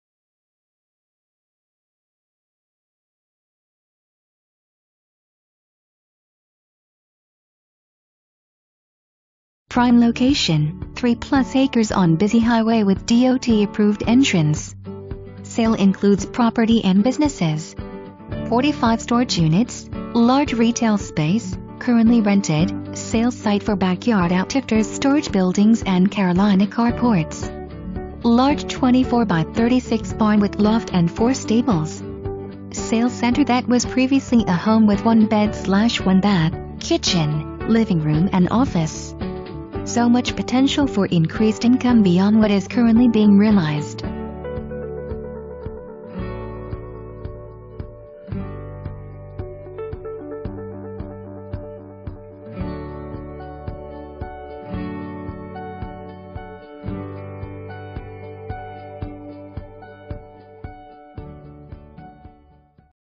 Coldwell Banker High Country Realty - 6850 APPALACHIAN HWY
6850 APPALACHIAN HWY
Blue Ridge, GA 30513
$1,490,000, MLS# 280399

PRIME LOCATION - Just a few feet from the entrance to the new UNG Campus sits 3+ acres on busy Hwy 515 with DOT approved entrance. Sale includes property and businesses. 45 storage units, Large retail space (currently rented), sales site for Backyard Outiftters Storage Buildings and Carolina Car Ports. Large 24 x 36 barn with loft and 4 stables - great for RV/boat storage. Sales Center that was previously a home with 1bed/1bath, kitchen, living room and office. SO MUCH POTENTIAL for increased income beyond what is currently being realized. Billboard is part of property and currently leased out.

Suzie Soave
Coldwell Banker High Country Realty